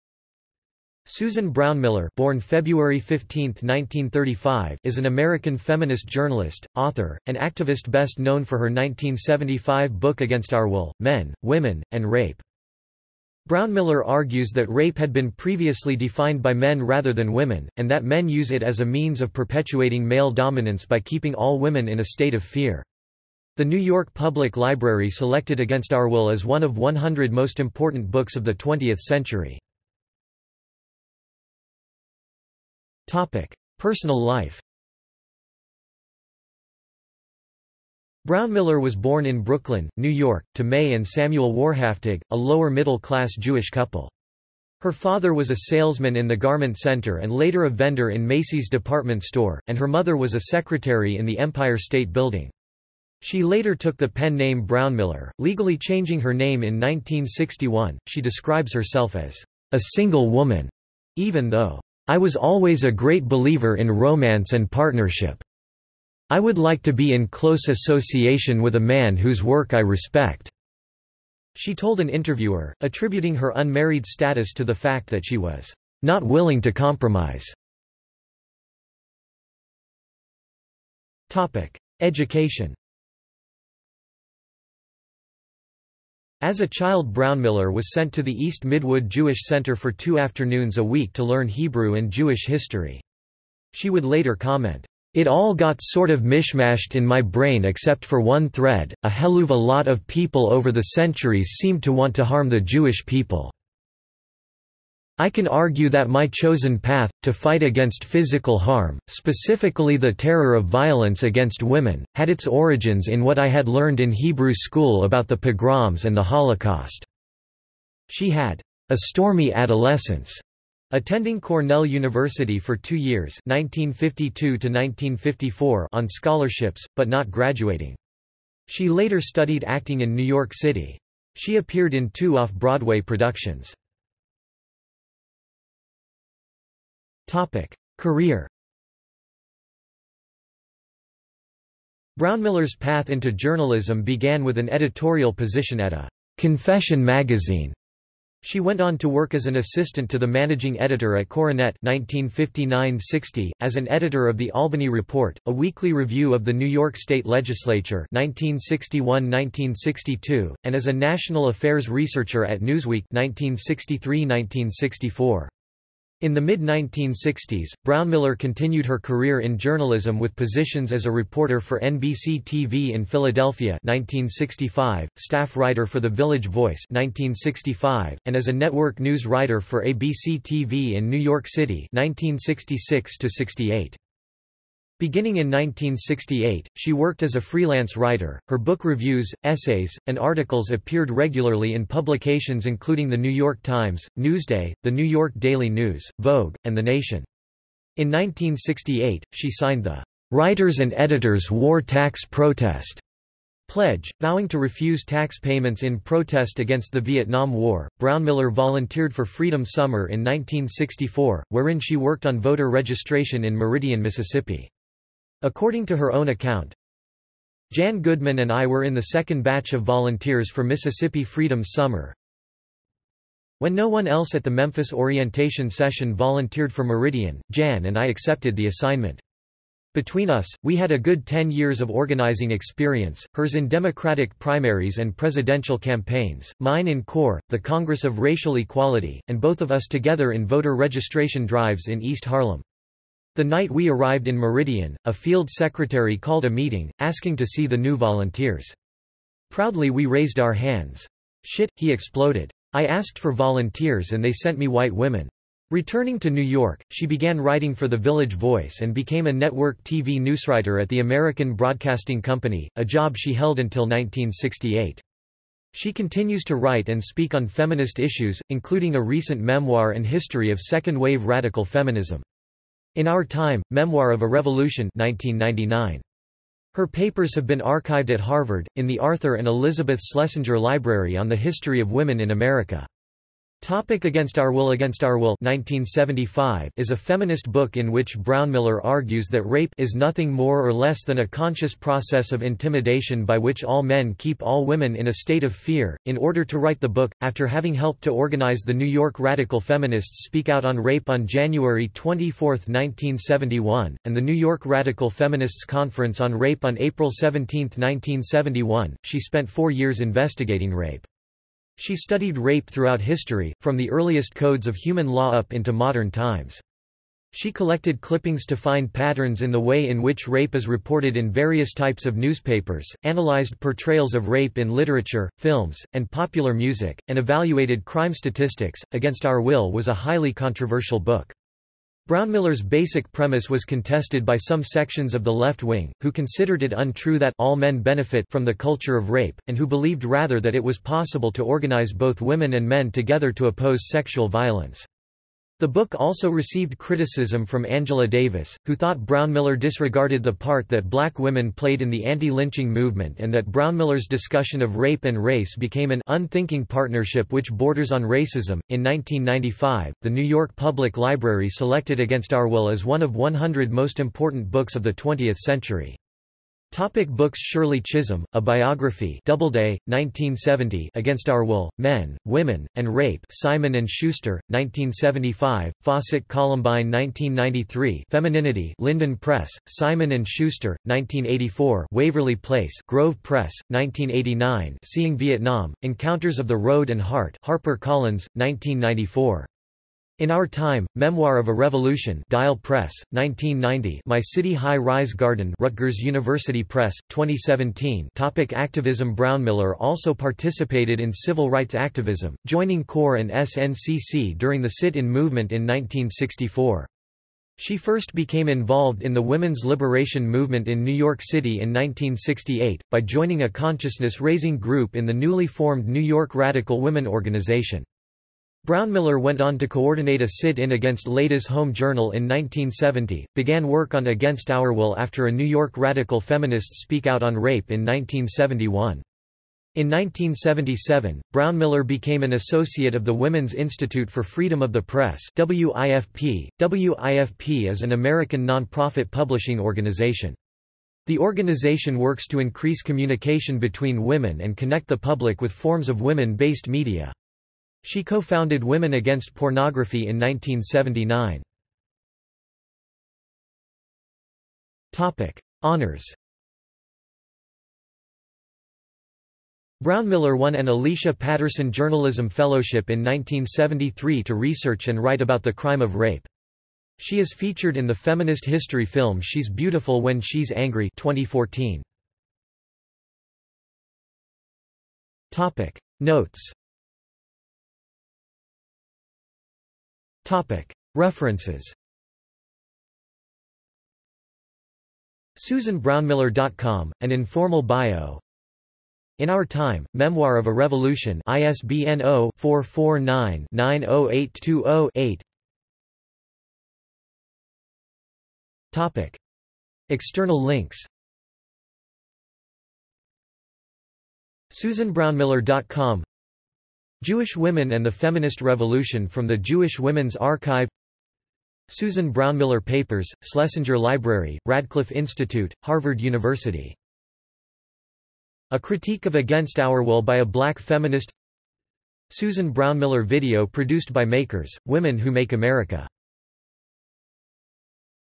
Susan Brownmiller | Wikipedia audio article
This is an audio version of the Wikipedia Article:
Susan Brownmiller

- Socrates

SUMMARY
Susan Brownmiller (born February 15, 1935) is an American feminist journalist, author, and activist best known for her 1975 book Against Our Will: Men, Women, and Rape.
Brownmiller argues that rape had been previously defined by men rather than women, and that men use it as a means of perpetuating male dominance by keeping all women in a state of fear. The New York Public Library selected Against Our Will as one of 100 most important books of the 20th century.